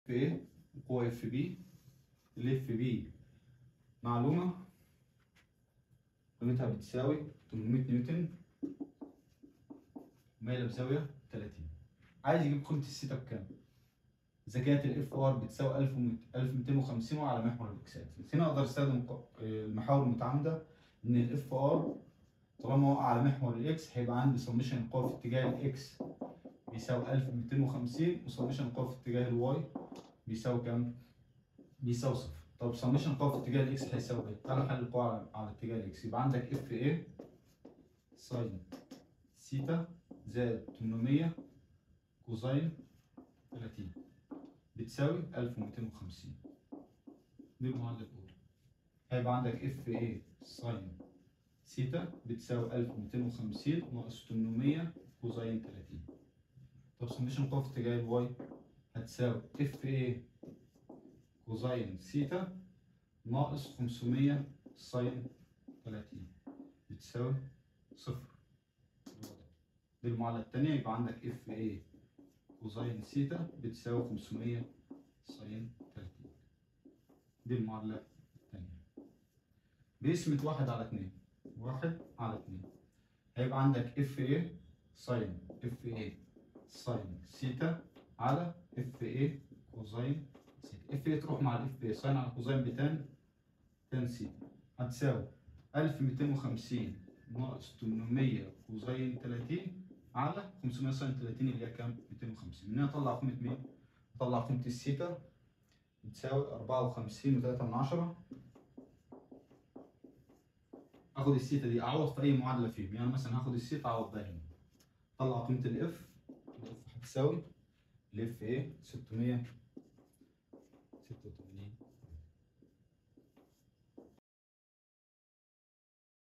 2-26.Determine the magnitude and direction 0 of FA So that the resultant force is directed
2-26. Determine the magnitude and direction 0 of FA So that the resultant force is directed along the positive x axis and has a magnitude of 1250 N
استاتيكا اعدادي هندسة